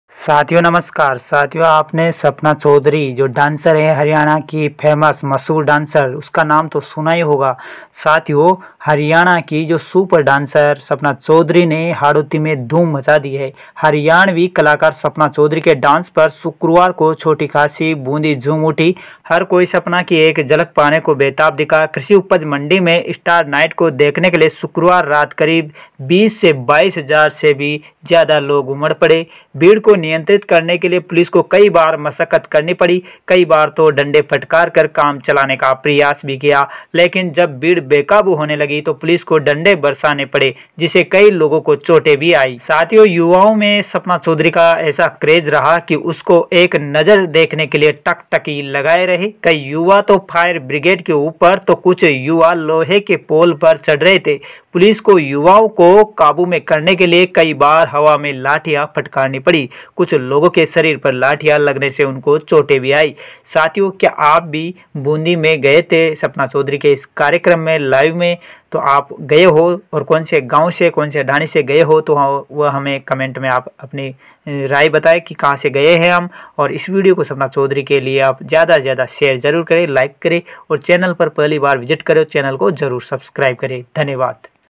सपना चौधरी के दीवानों को बूंदी मंडी में भगा-भगा के मारा | Sapna Chaudhari Bundi Mandi
सपना चौधरी ने छोटी काशी बूंदी में धूम मचाई | हाडोती के लोगों को संभालना हो गया मुश्किल